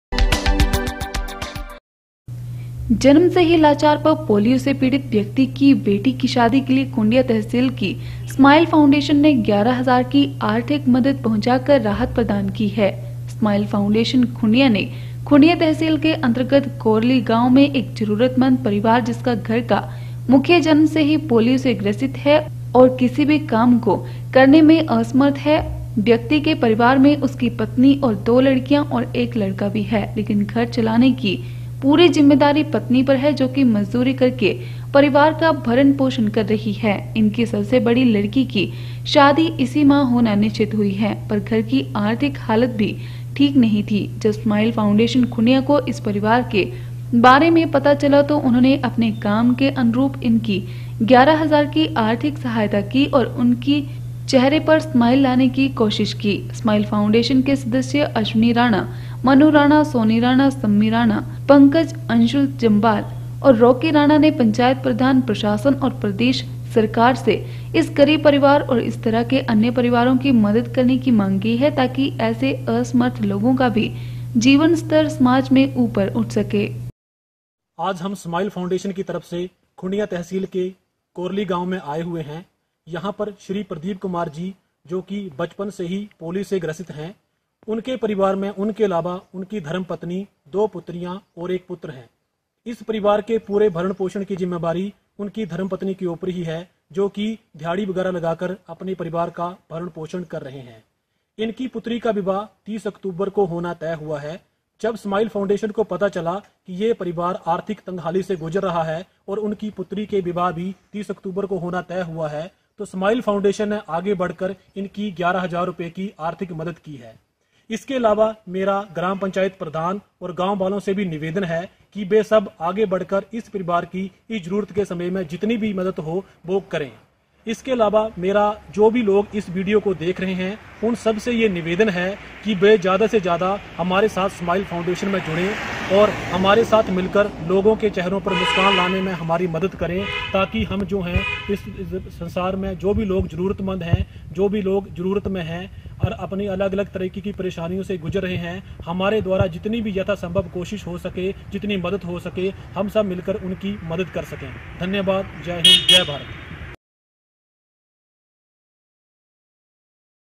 2 NOV 2 पोलियो पीड़ित व्यक्ति की बेटी की शादी के लिए स्माइल फाउंडेशन ने पहुंचाई 11 हजार की आर्थिक मदद
जन्म से ही लाचार व पोलियो से पीड़ित व्यक्ति की बेटी की शादी के लिए खुंडिया तहसील की स्माइल फाउंडेशन ने 11 हजार की आर्थिक मदद पहुंचा कर राहत प्रदान की है।स्माइल फाउंडेशन खुंडिया ने खुंडिया तहसील के अंतर्गत कोरली गाव में एक जरुरतमंद परिवार जिसका घर का मुखिया  जन्म से ही पोलियो से ग्रसित है और किसी भी काम को करने में असमर्थ है।